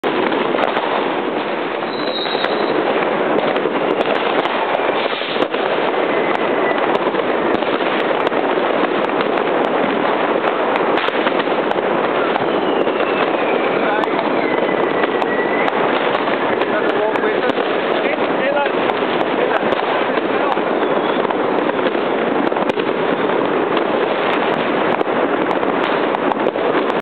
Fireworks in Amsterdam New Years 2010
Fireworks going off in Oud Zuid in Amsterdam, New Years Eve 2010.